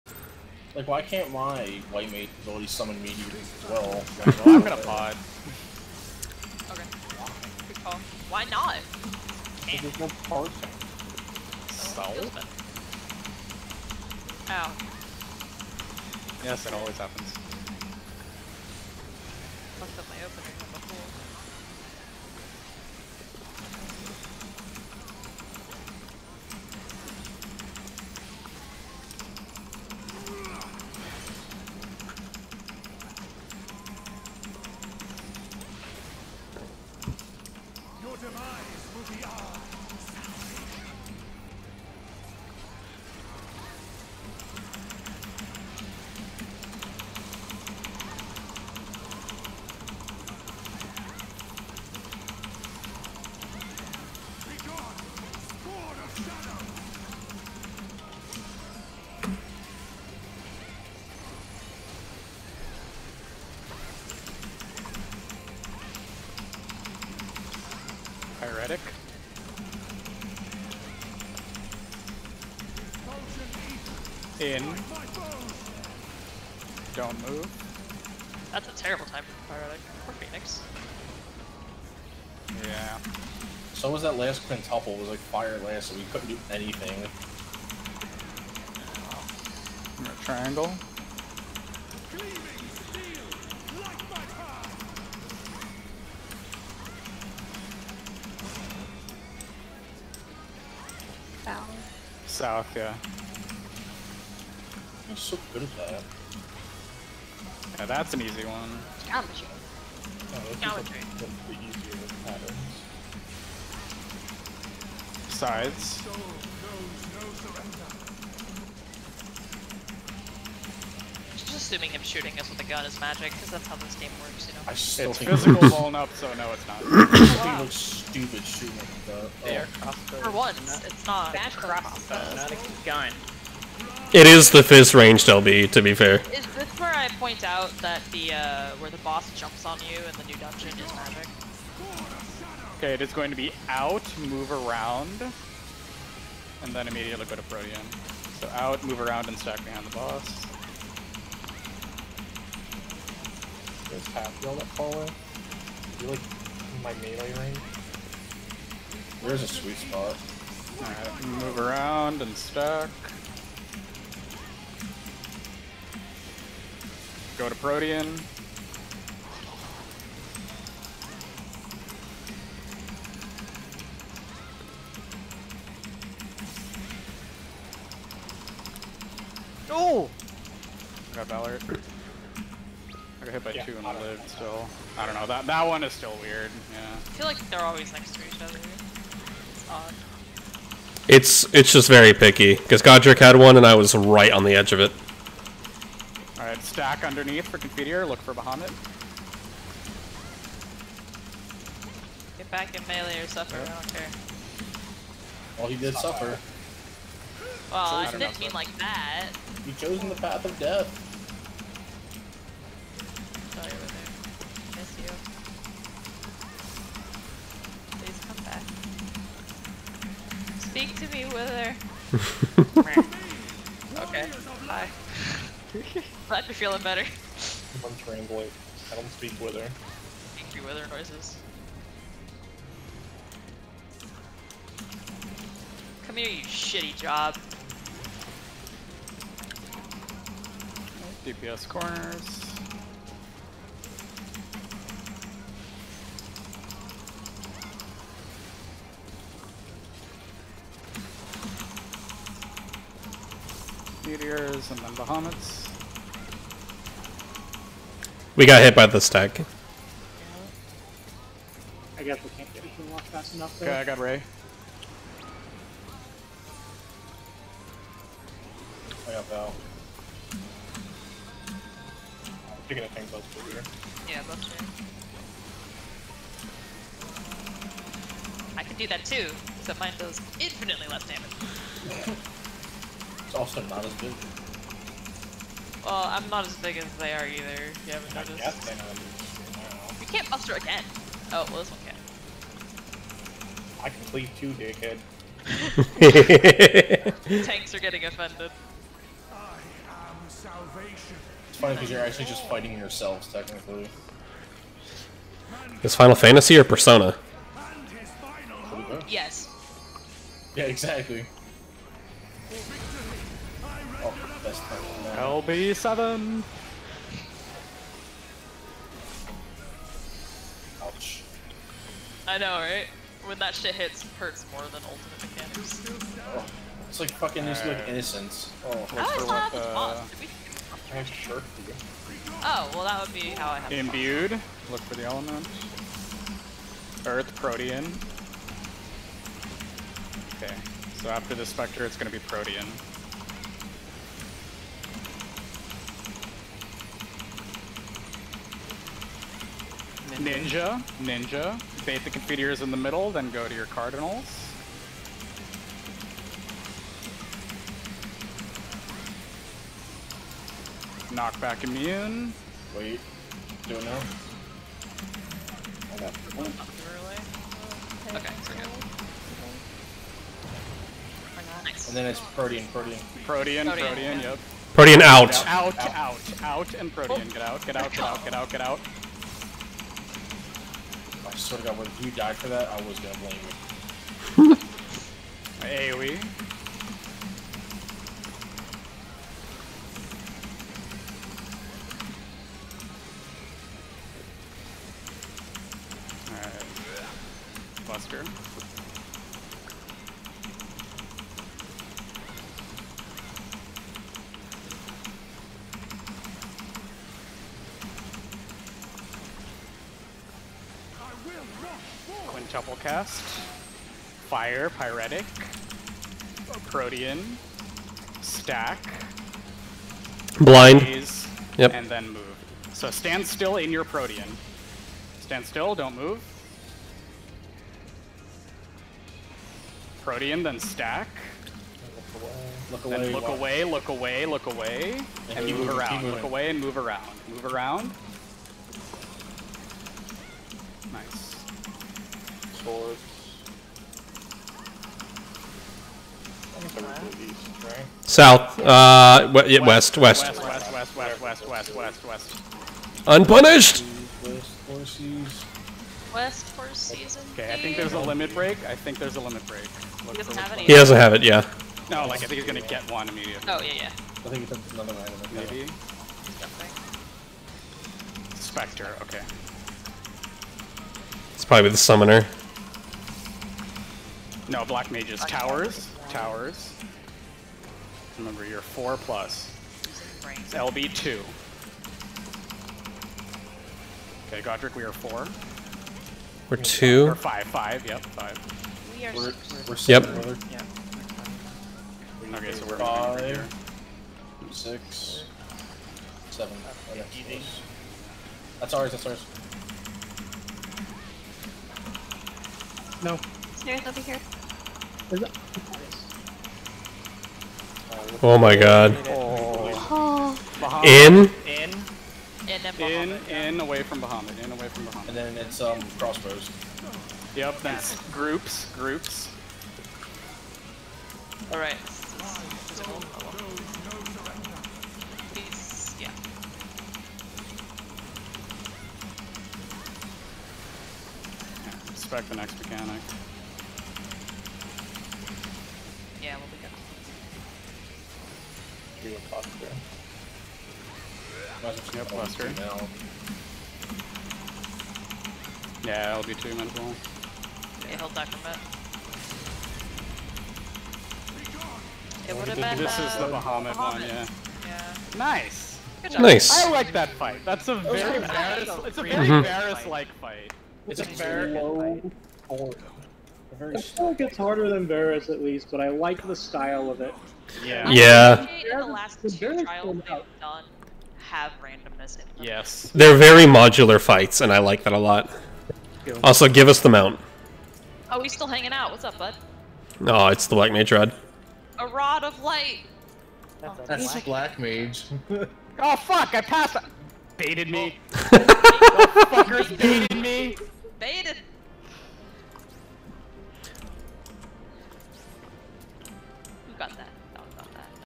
5.3 EX Trial Clear MCH POV (loud audio)
Sorry about the audio, it was boosted from a prior mic setup I used on a different stream.  Nevertheless, this was still a great fight with lots of modularity!  I'm really digging this kind of fight design.